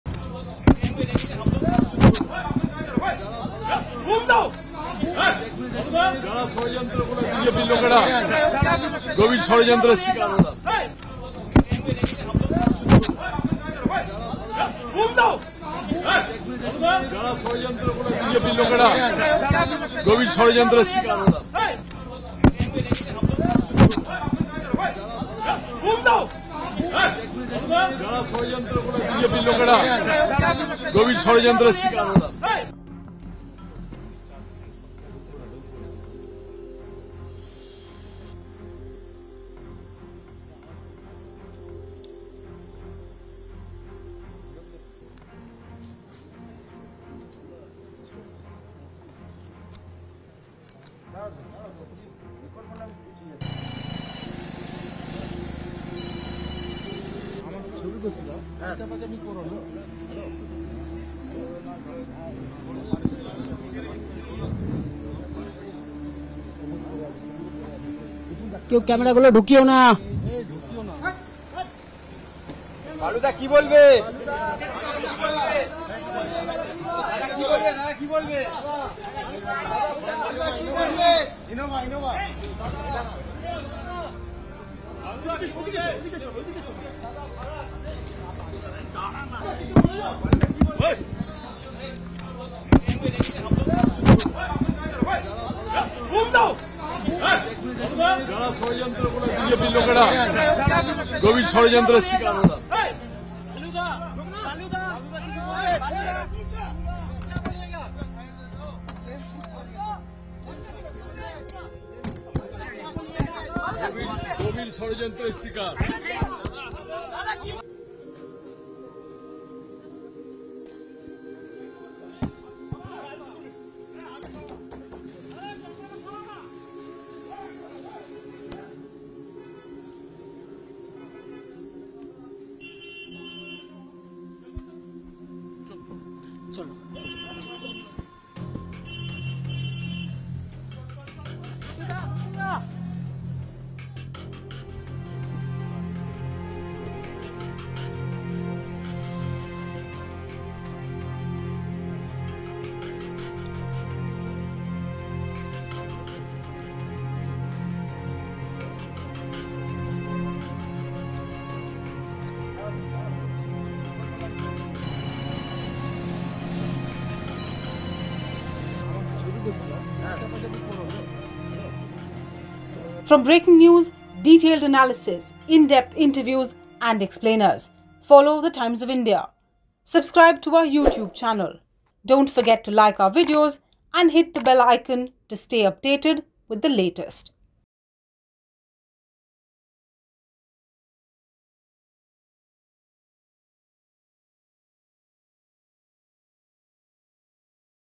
ED arrests TMC minister Jyotipriya Mallick in Ration Scam | Victim of great conspiracy, says Mallick
The Enforcement Directorate (ED) has arrested West Bengal's current Forest Minister and former Food and Supplies Minister, Jyotipriya Mallick, in connection with a multi-crore ration distribution case. Following an extensive 20-hour raid at Mallick's residence in Salt Lake, Kolkata, he was brought to the ED's central government office complex in Salt Lake around 4 a.m. on Friday. Initially, it was unclear whether he had been arrested or was being questioned further. However, by 6 a.m., it was confirmed that Mallick had indeed been arrested. Upon his arrival at the ED office, Mallick alleged that he was a victim of a conspiracy orchestrated by the BJP. He becomes the second West Bengal cabinet member, after Partha Chatterjee, to be arrested in connection with an alleged money laundering scandal. Furthermore, he is the fourth TMC legislator, following Chatterjee, Manik Bhattacharya, and Jiban Krishna Saha, to be taken into custody by the central agency.